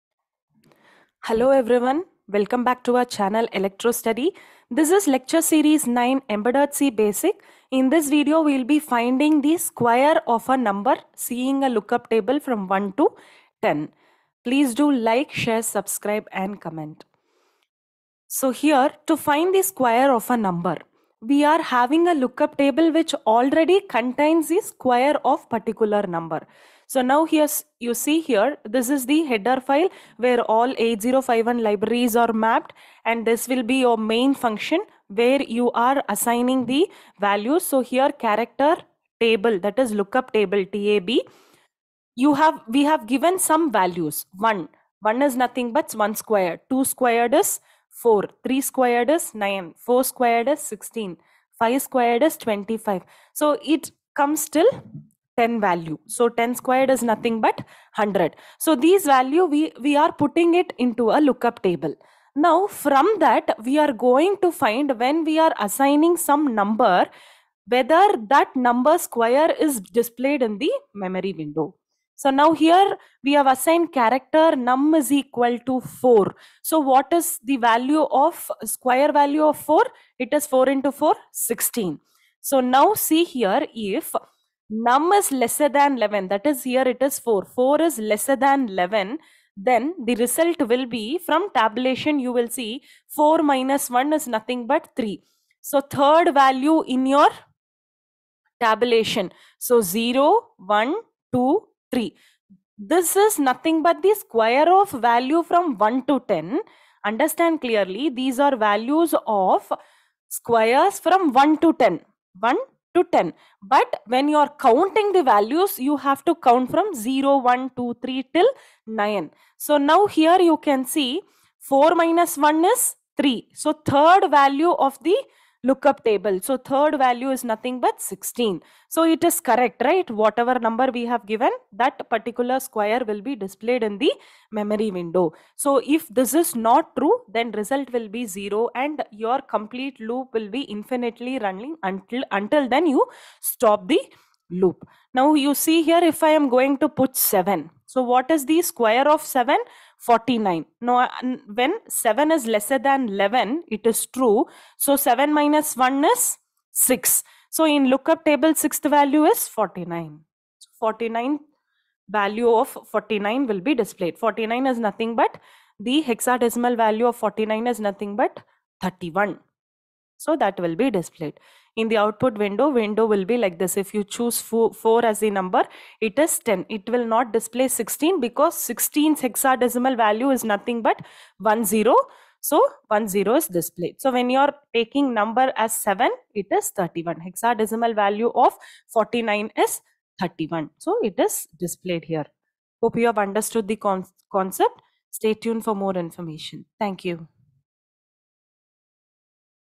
Square of a number from look-up table|Embedded C Basics|BE|VTU|4th Sem|ECE|CS|8051 Microcontroller
Hi all, In this video we will be seeing about the Squaring of a number from a look up table.